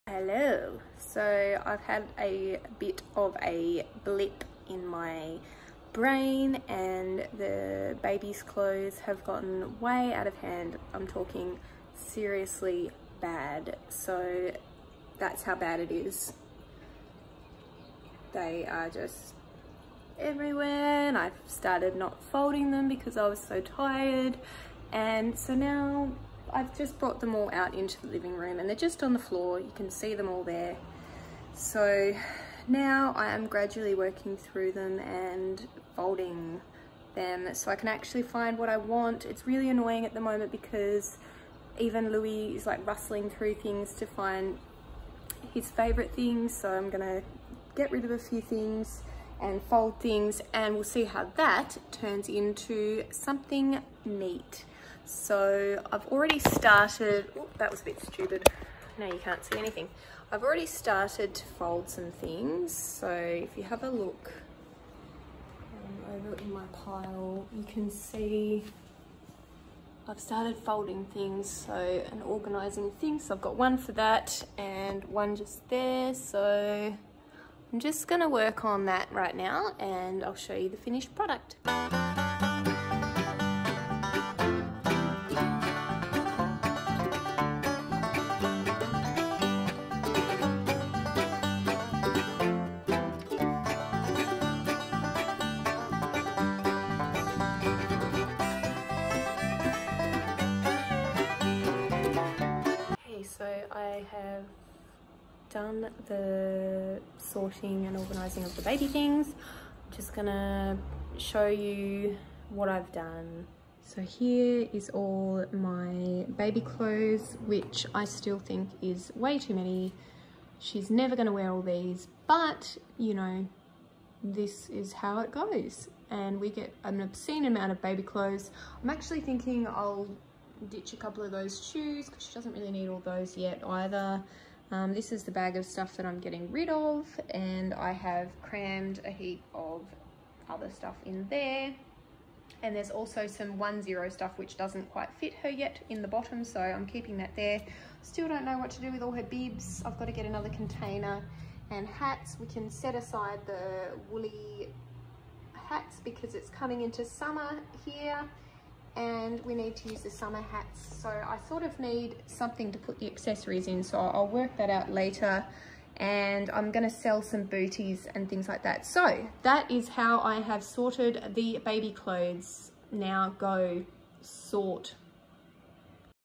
Sorting and folding baby clothes. Go on, you know you want to!!!
Hey I’ve really let the baby clothes get out of control to the point where I was just shoving things anywhere and all over the floor in our bedroom. We couldn’t find anything! I’ve folded and sorted them all out. Check out how I did it